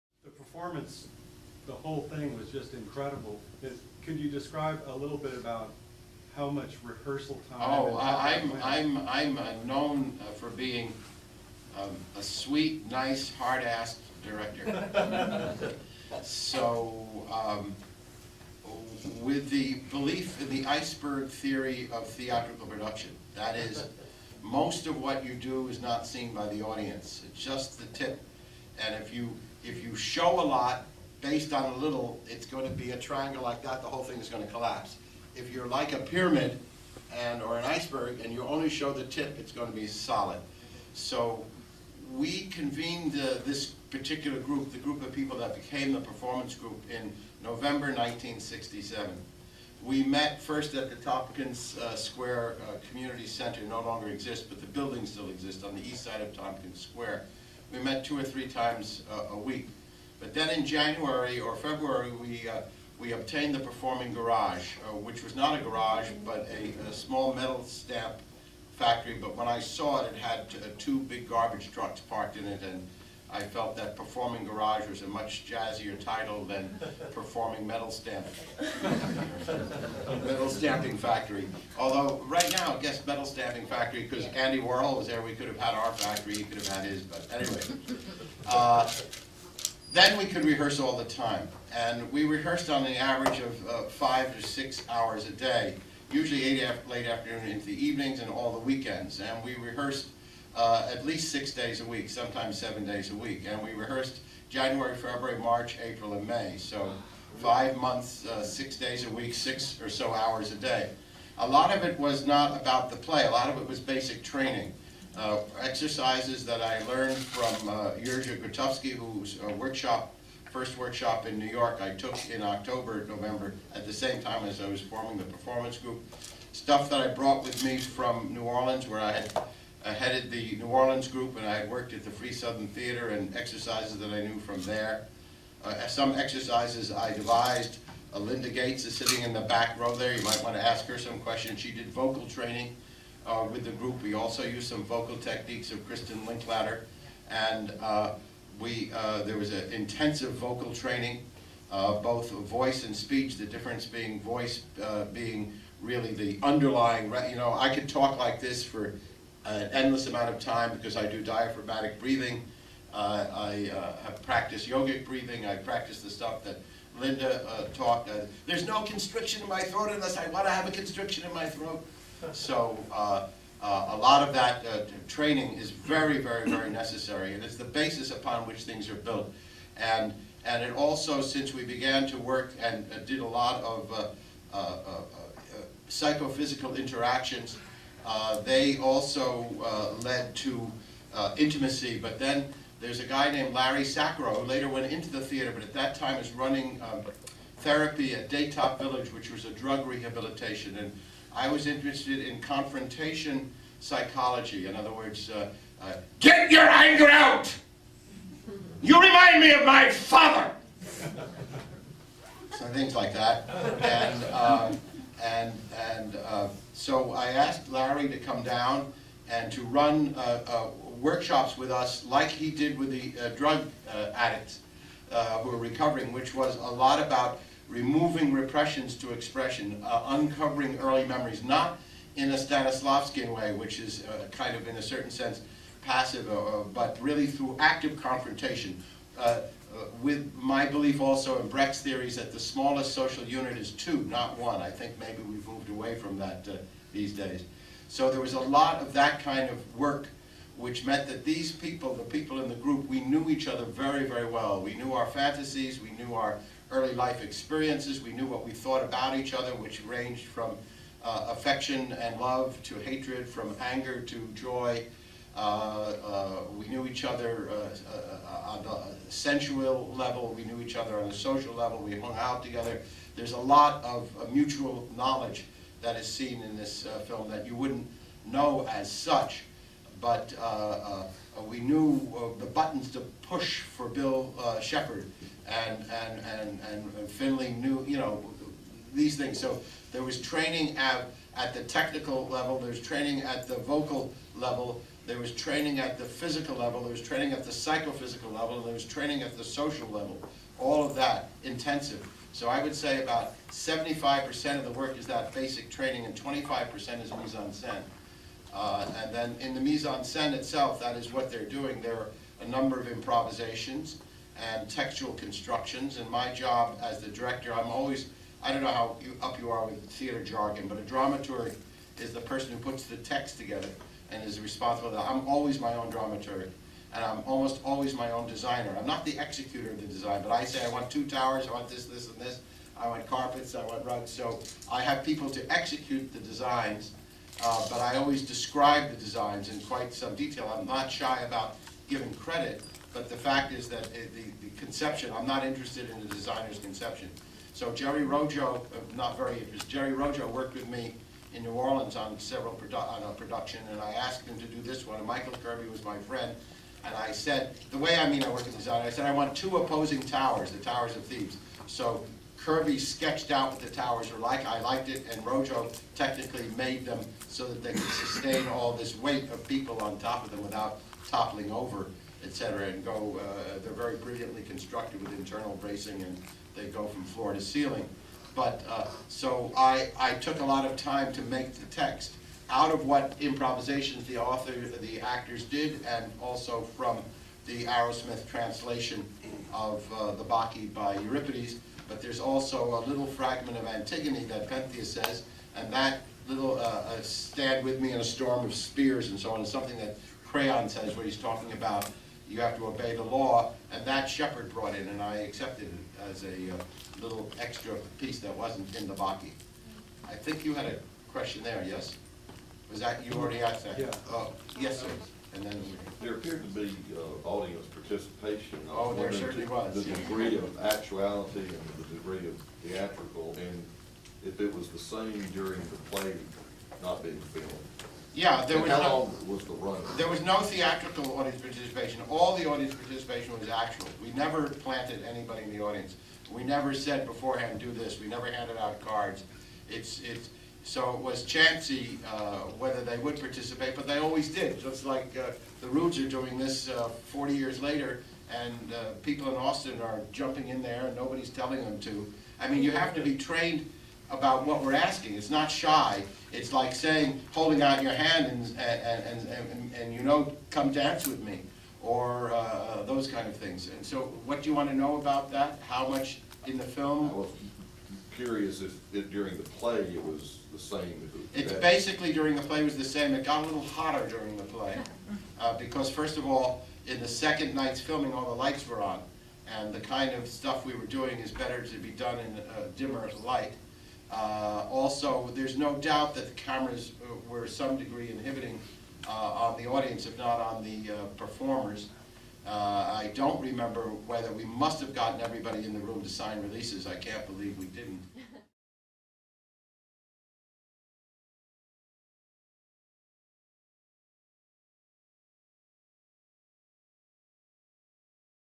Performance Director Richard Schechner Talks about DIONYSUS IN '69 Part Two
Austin Film Society & Rude Mechanicals present performance director Richard Schechner discussing DIONYSUS IN '69 after screening at Alamo Ritz on Dec. 6 (part two).  Rude Mechanicals will be performing DIONYSUS IN '69 Thursdays through Sundays Dec. 3 - 20, 2009.
(posted by vid/ed Anne Heller)